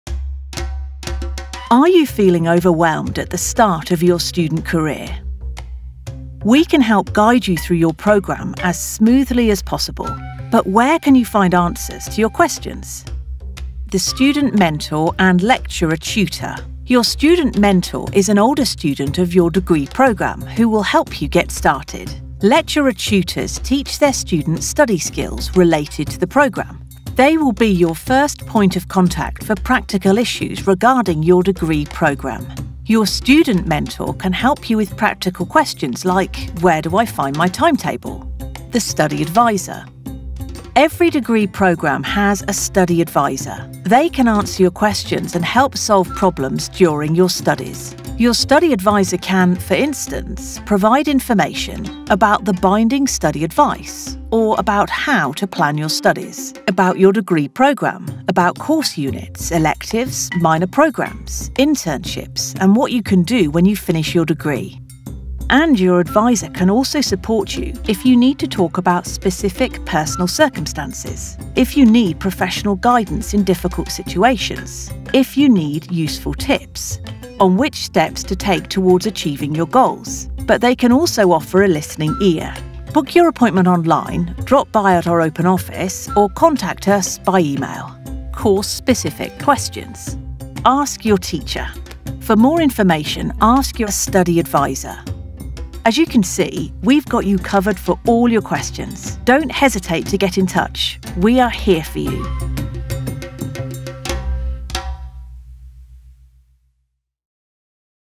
Study Support
In this short video, you will find information about Study Support at the Faculty of Arts, University of Groningen.